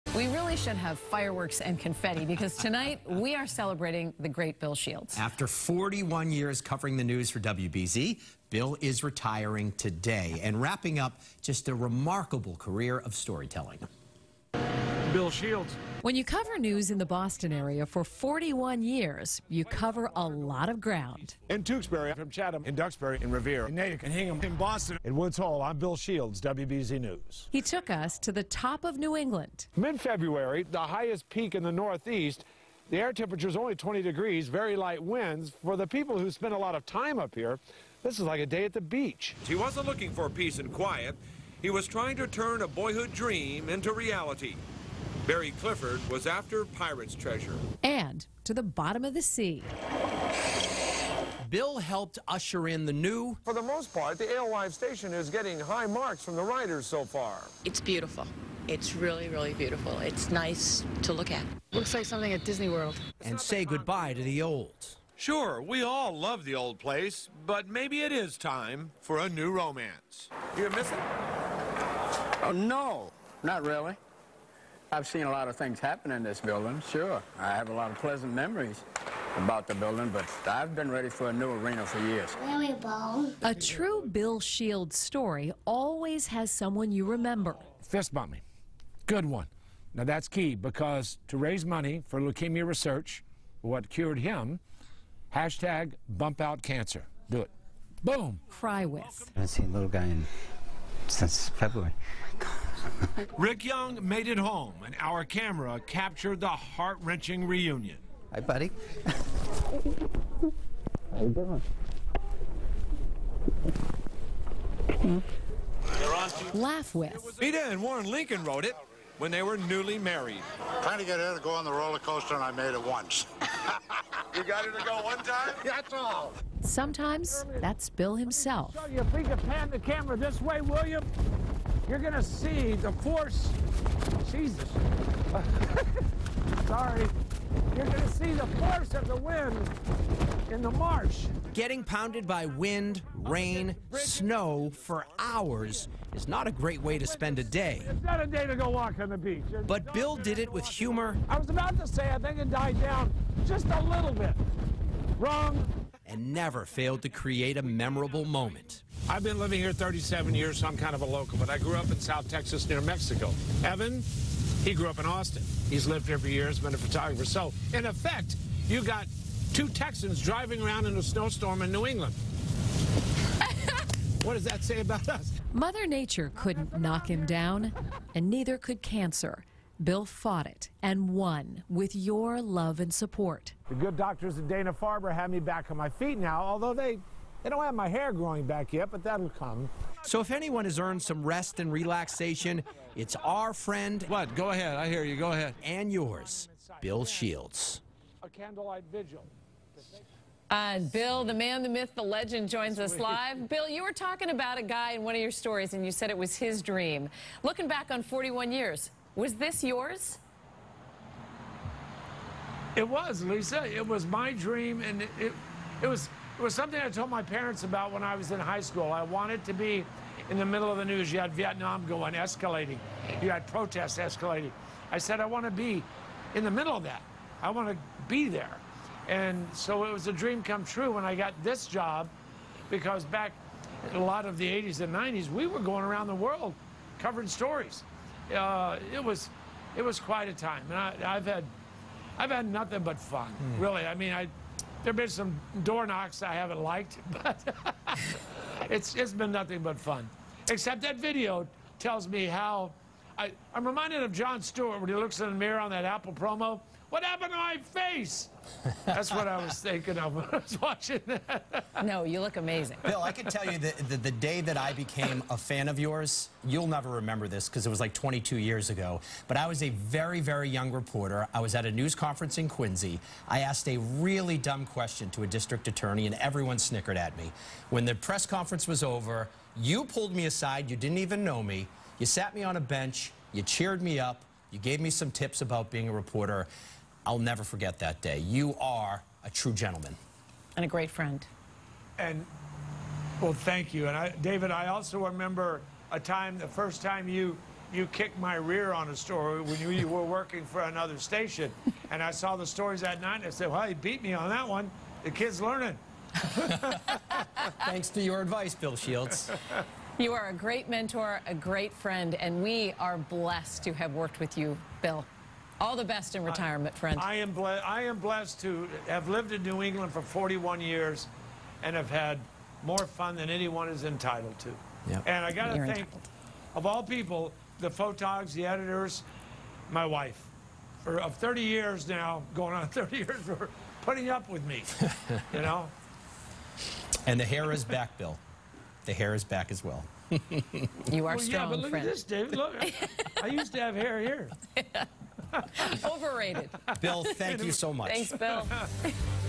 Bill Shields Retires After 41 Years At WBZ
After 41 years covering the news for WBZ, Bill Shields has retired, wrapping up a remarkable career of storytelling.